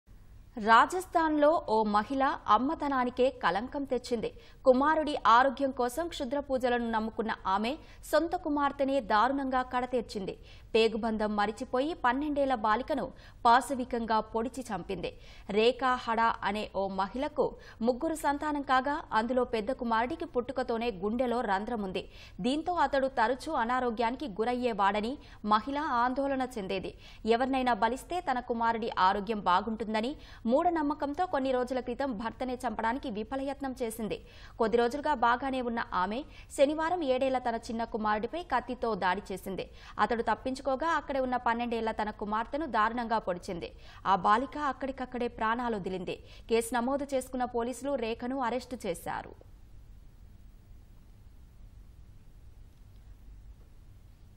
12 Yr Old Girl Brutually Killed by Mother in Rajasthan | Human Sacrifice for Son
రాజస్థాన్ లో ఓ మహిళ. అమ్మ తనానికే కలంకం తెచ్చింది. కుమారుడి ఆరోగ్యం కోసం...క్షుద్రపూజలను నమ్ముకున్న ఆమె సొంతకుమార్తెనే దారుణంగా కడతేర్చింది. పేగు బంధం మరచిపోయి 12 ఏళ్ల బాలికను పాశవికంగా పొడిచి చంపింది. రేఖా హడా అనే ఓ మహిళకు ముగ్గురు సంతానం కాగా. అందులో పెద్ద కుమారుడికి పుట్టుకతోనే గుండెలో రంధ్రం ఉంది. దీంతో అతడు తరచూ అనారోగ్యానికి గురయ్యేవాడని మహిళ ఆందోళన చెందేది. ఎవర్నైనా బలిస్తే తన కుమారుడి ఆరోగ్యం బాగుంటుందని మూఢనమ్మకంతో కొన్ని రోజుల క్రితం భర్తనే చంపడానికి విఫలయత్నం చేసింది. కొద్ది రోజులుగా బాగానే ఉన్న ఆమె ...శనివారం ఏడేళ్ల తన చిన్న కుమారుడిపై కత్తితో దాడి చేసింది. అతడు తప్పించుకోగా అక్కడే ఉన్న 12ఏళ్ల తన కుమార్తెను దారుణంగా పొడిచింది. ఆ బాలిక అక్కడికక్కడే ప్రాణాలొదిలింది. కేసు నమోదు చేసుకున్న పోలీసులు రేఖను అరెస్టు చేశారు..